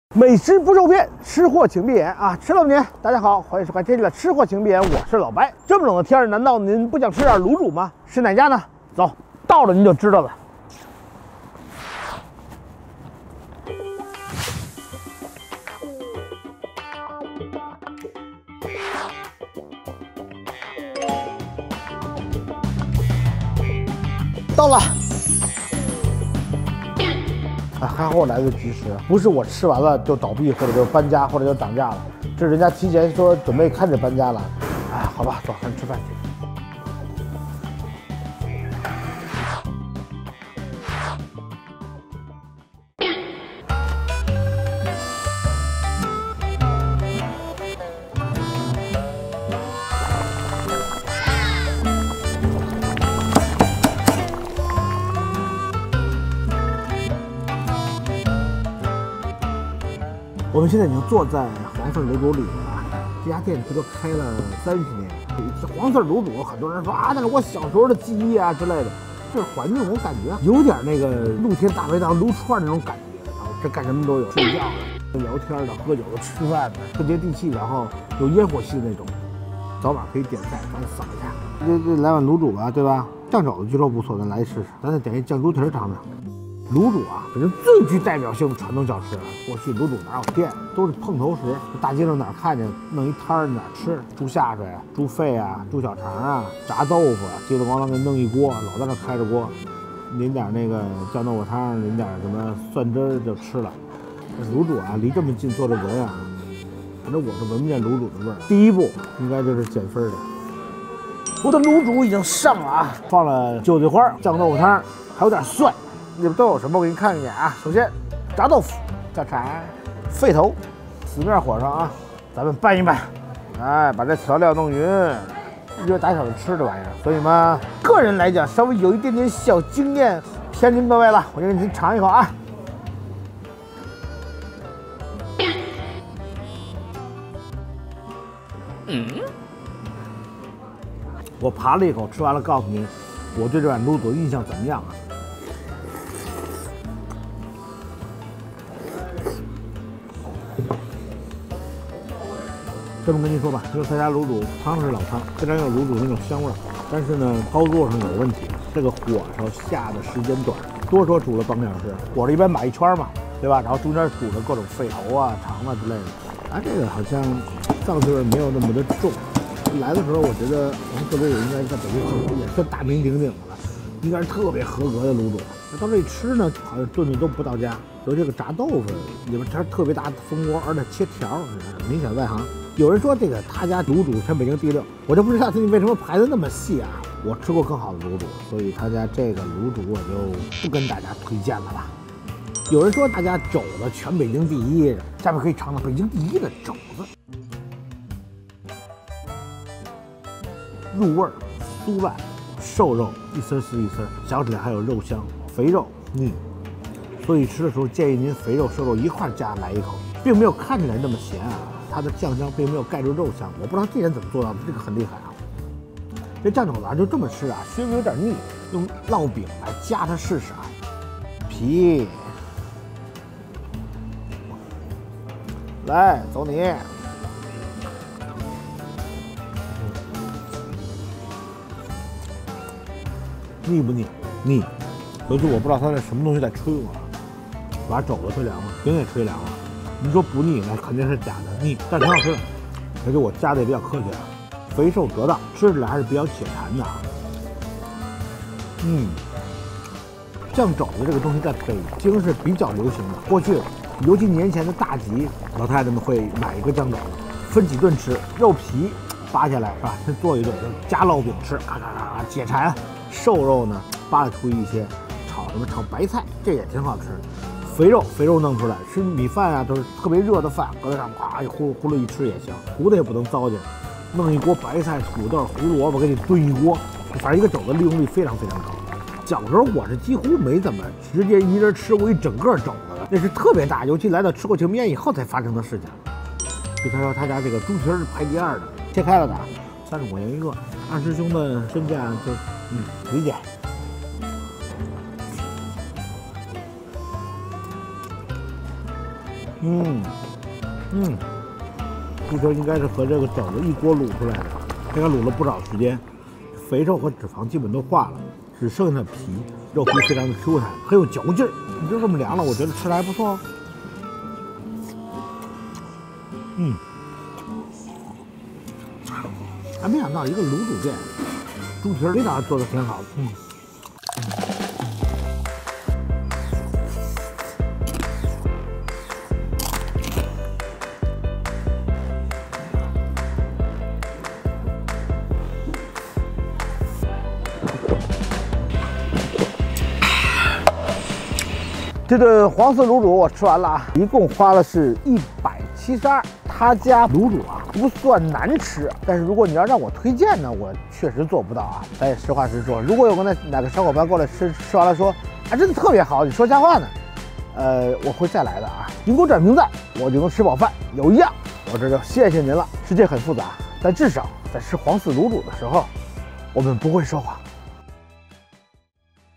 【吃货请闭眼】藏在地下的北京第一酱肉？80元一个肘子软烂入味，配烙饼吃超香！【Justeatit Official Channel】
【吃货请闭眼】不说谎的美食探店短视频
   每周二、五努力更新，爱我就赞我吧！

老北京卤煮，搭配烙饼倍儿香，冬天就要吃口热乎的！